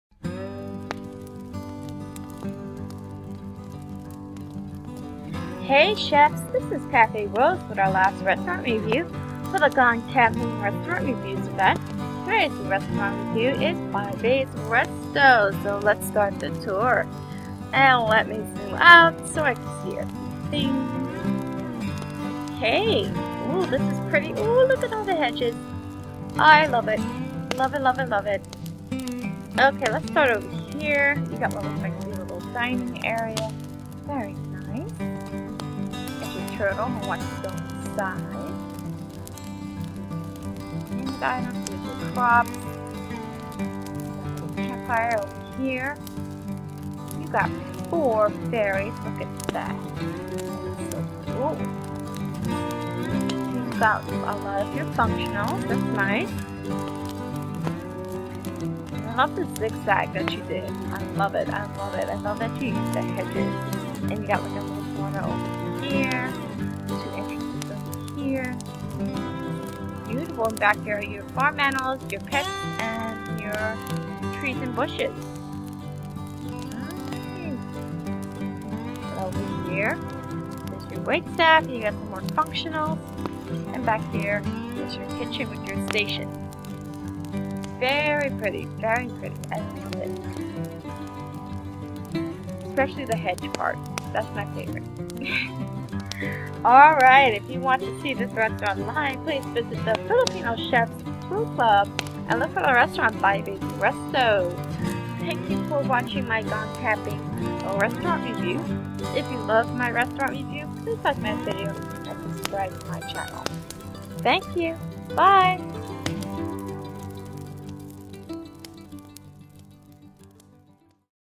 Gone Camping Restaurant Reviews - Santos
Hey, Chefs! Today's Gone Camping restaurant review is Baibes Resto of the Filipino Chefs food club.